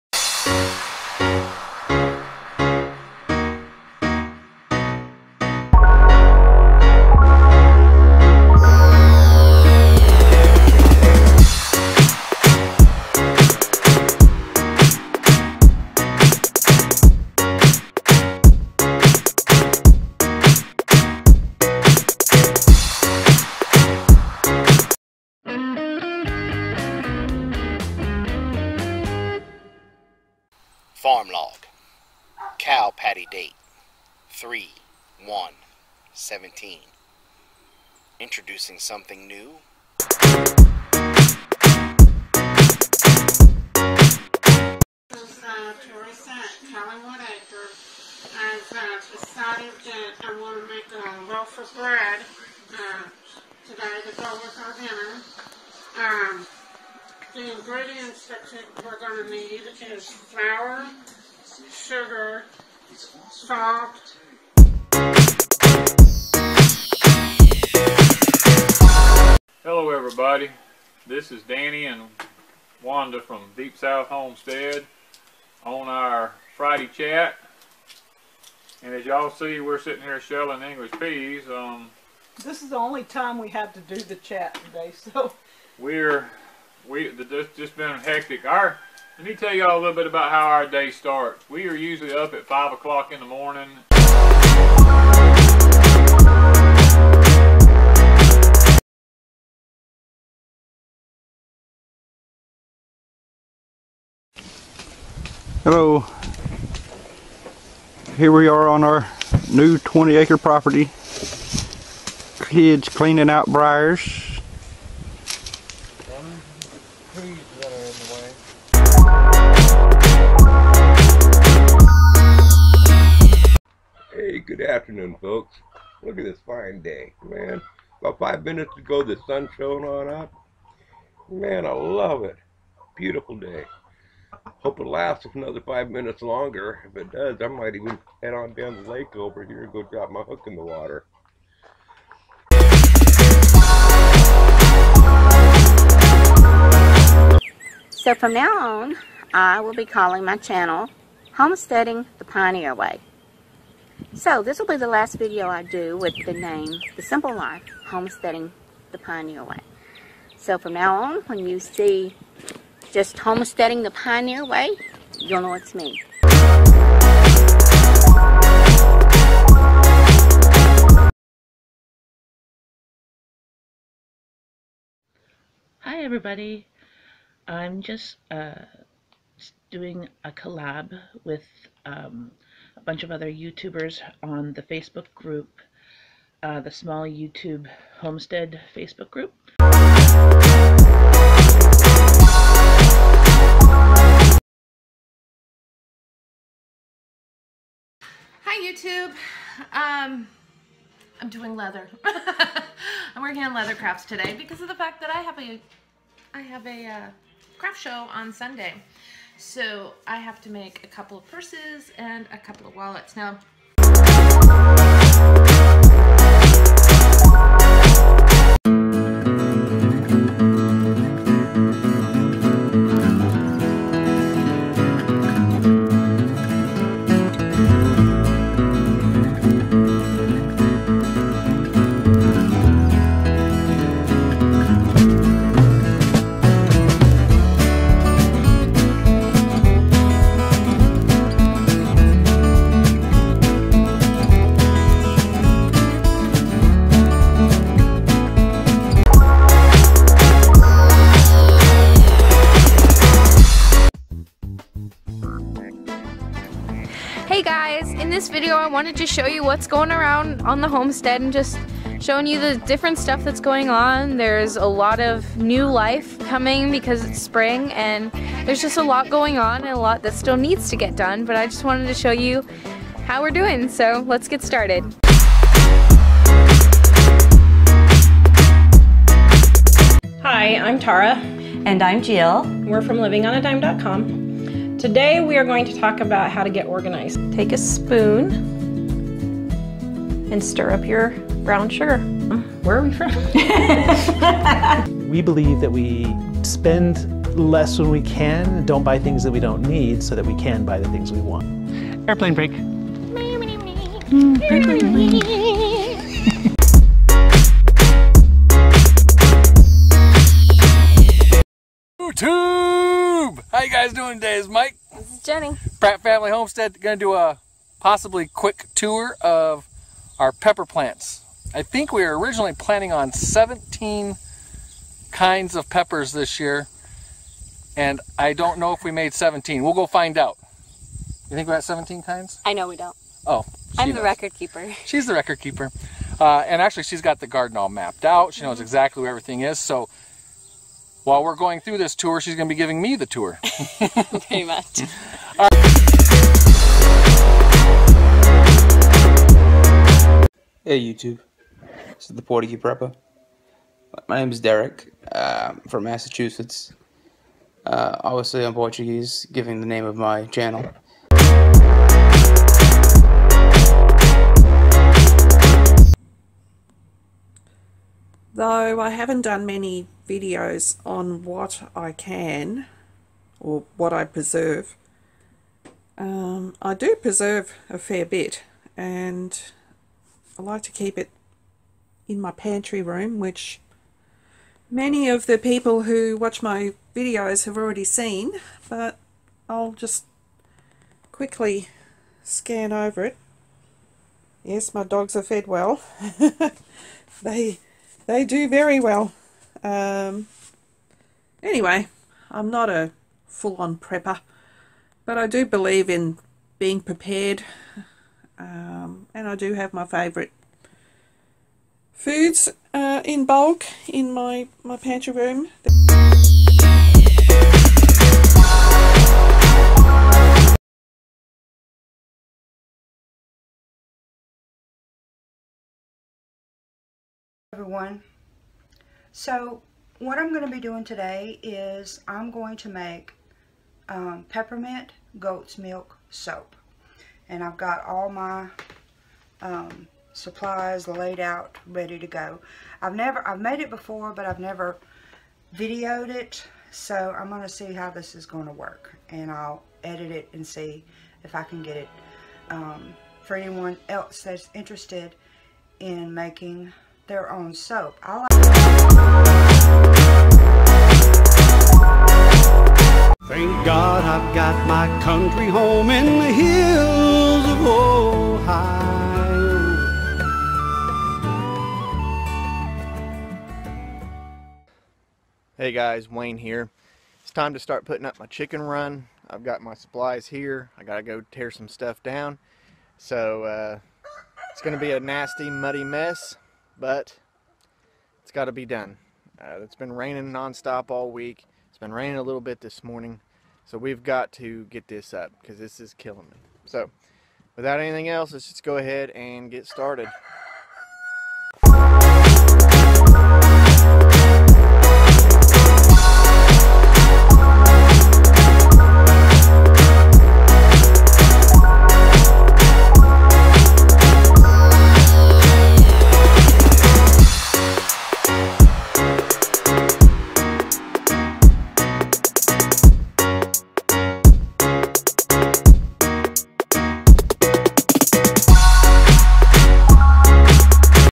Collab Participants - 4/2/17
This video was created the afternoon of 4/2/17 so if you joined the challenge after that, it's not like we forgot you :) If you do your own live streams maybe you could inform folks HEY join me in a challenge I'm doing over at TwoFamilyHomestead and all they'd have to do is watch our collaboration/challenge video.  REMEMBER you have to post "I'm in" in the comments on the original video :)